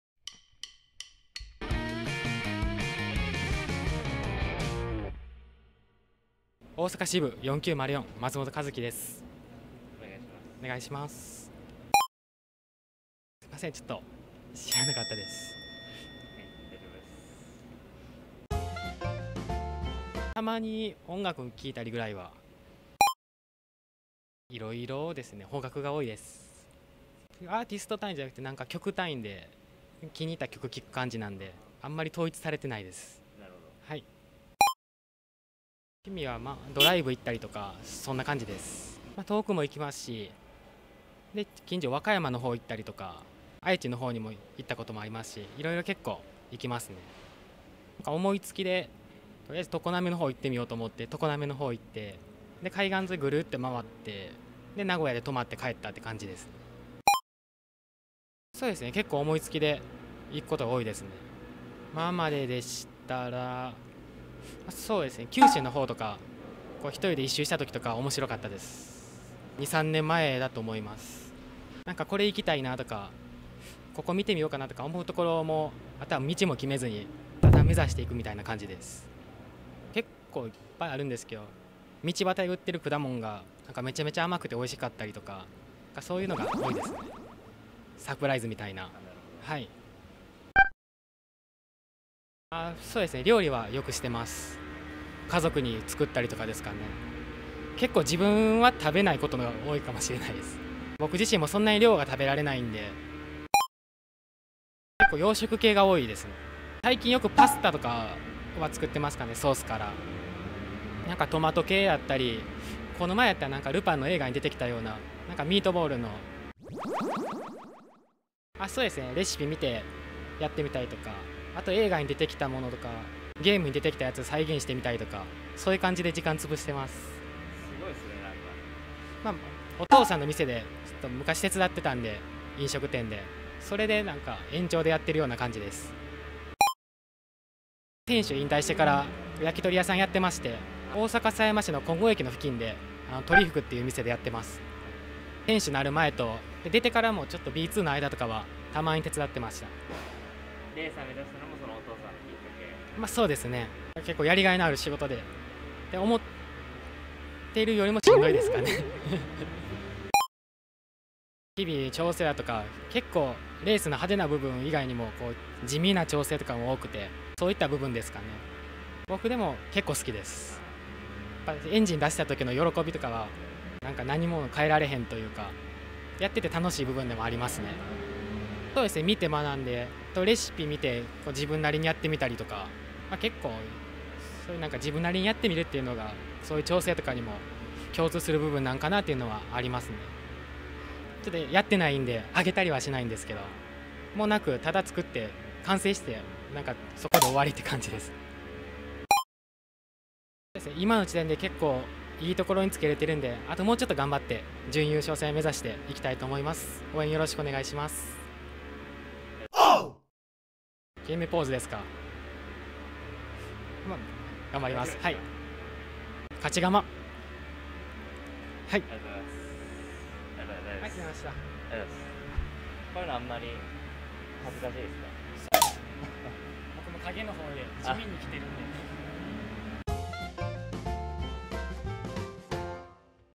【蒲郡】「趣味は創作料理とひとり旅」松本一毅選手にインタビュー！【ルパンのパスタ】
【三遠ネオフェニックス杯争奪e-じゃんカードカップ】4日目
大阪支部、松本一毅選手にインタビューしてきました！
大阪の人ごめんなさい、クセの強い人ばかりだと思ってましたがなんという好青年。
優しくとてもしっかりと質問に答えてもらいました！
ちなみに、松元選手のお父さんは引退した3263松本浩一さんです。
現在は焼き鳥屋さんをされているそうですよ！

vol.2246

電話投票会員様向け蒲郡勝ガマキャッシュバックキャンペーンサイト